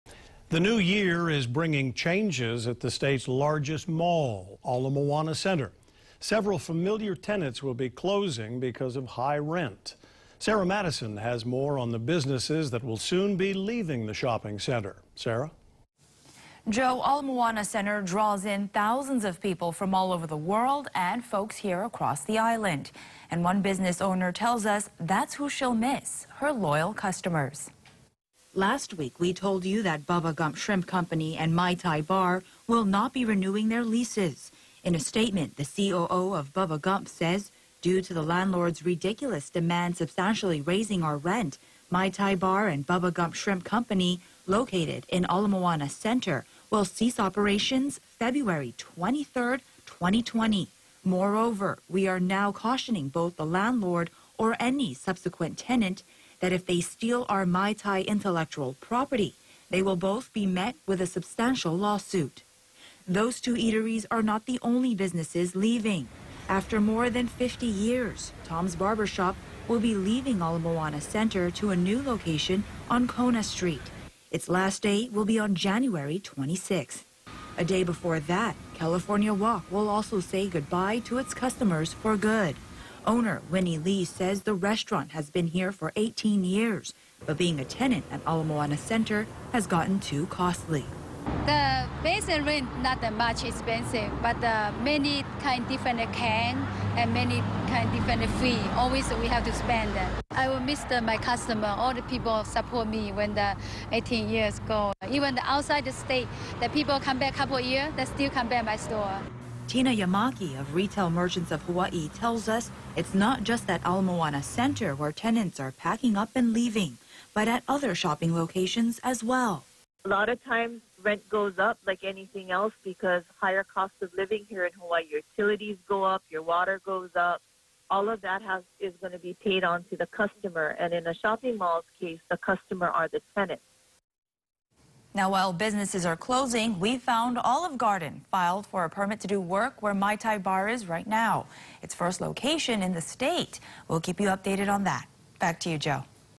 Ala Moana Center tenants closing doors citing high cost in rent
A new year brings changes at the state's largest mall Ala Moana Center. Several familiar tenants will be closing because of high rent.